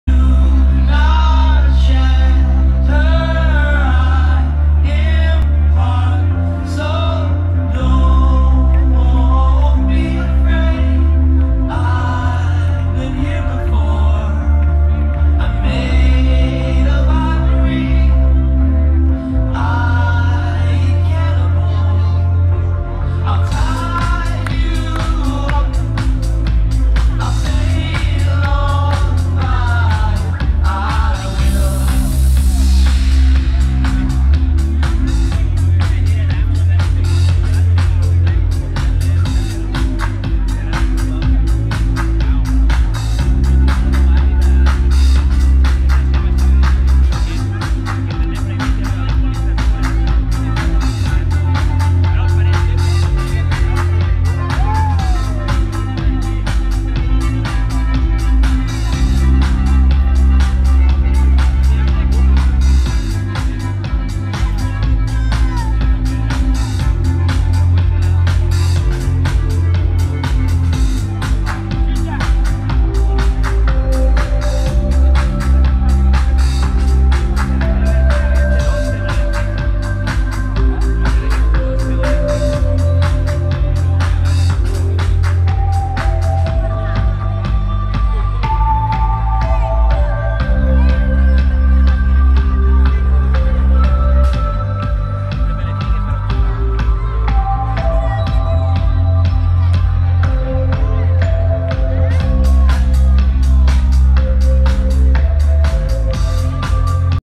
Pillow Talk live @ Kappa FuturFestival 2015
Pillow Talk (live) - Day 2 of KFF15 @ Parco Dora, Torino, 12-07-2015  -  DORA STAGE